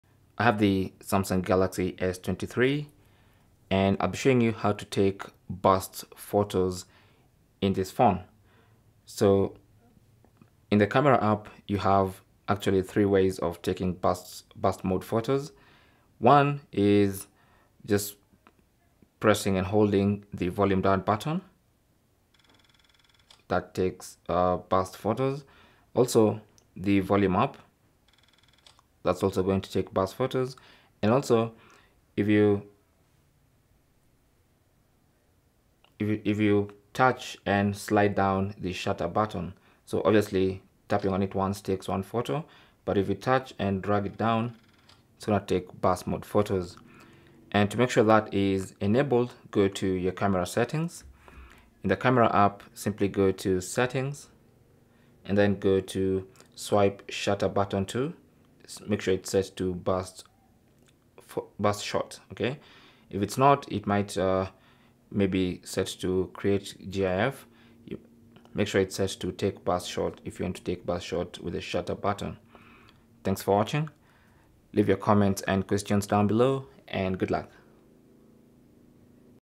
How To Take Burst Photos On Samsung Galaxy S23/S23+/S23 Ultra | Burst Mode
Learn how to capture burst photos on Samsung Galaxy S23, S23+, and S23 Ultra.
Burst photo is a mode whereby several photos are captured in quick succession by either pressing the shutter button or holding it down. This feature is found in newer smartphones.

Once you capture the photos, you might choose to keep the ones that you find good. There are a couple of ways you can use to capture burst photos on the Samsung Galaxy S23.

Launch the camera application on your Samsung Galaxy S23. Press and hold the volume buttons to capture burst photos. Once you've captured enough, simply release the button. Another way to capture burst photos on Samsung S23 is by using the shutter button.

Press and hold the shutter button and slide down to capture. Release the shutter button once you've captured enough photos.

Cell Phone Tripod Adapter